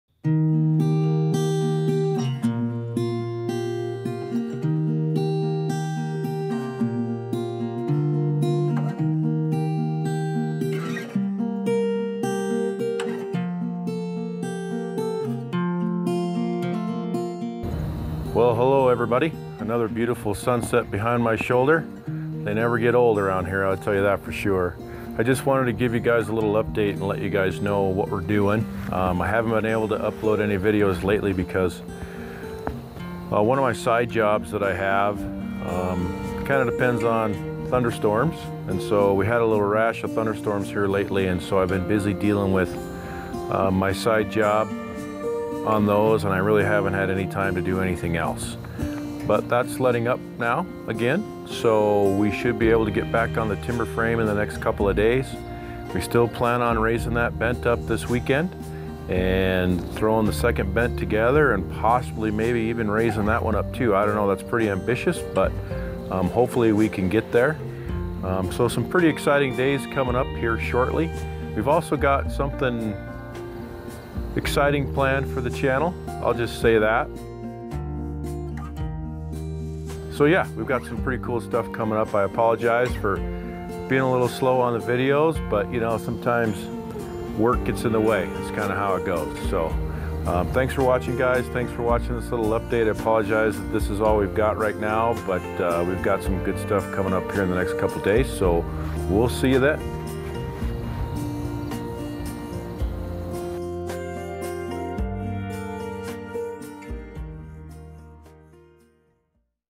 Timber Frame Barn Update
Here is an update on the timber frame barn.  I have been busy with work for a while but we have lots coming up for the weekend!